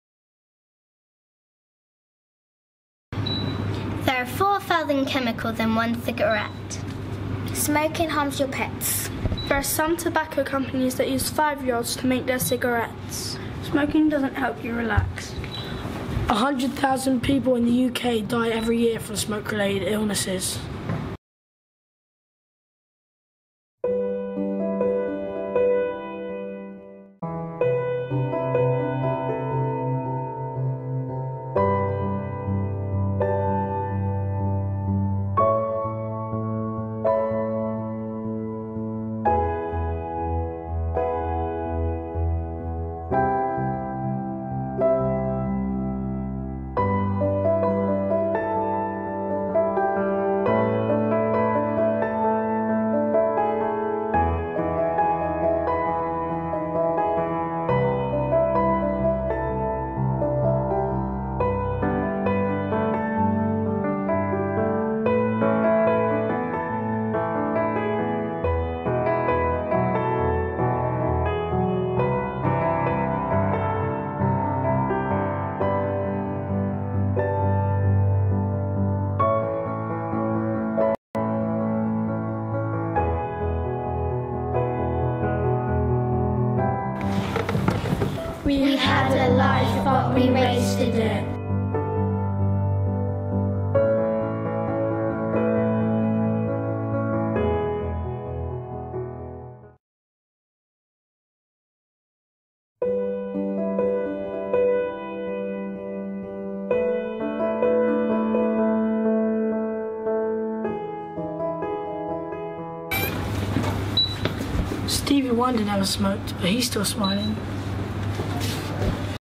Smoking Kills
A film on how smoking kills